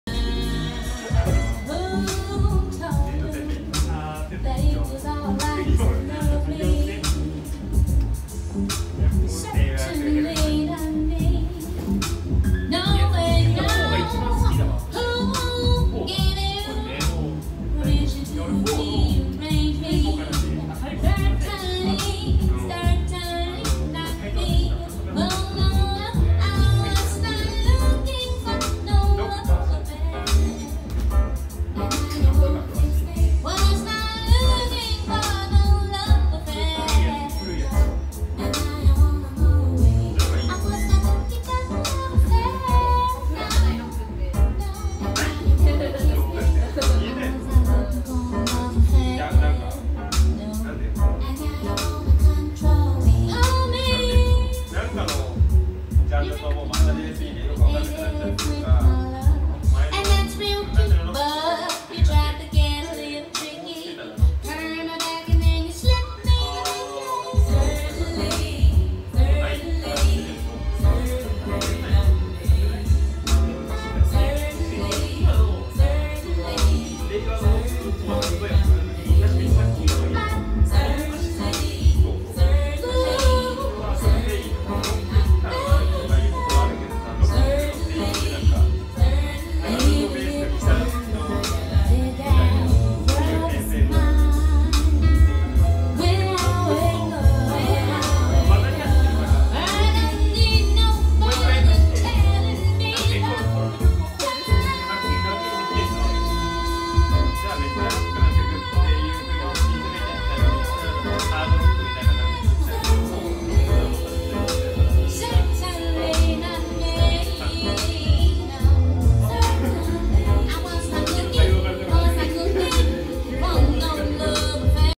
DJ III FOOT at 神保町・レコードバー
神保町レコードバー
JBC  Record Bar
東京都千代田区神田神保町1-12-6-B1

2011年11月8日月曜日

We've got the jazz. イベント
　毎月第2月曜日開催

エリカ・バトウー の楽曲主の箇所を収録。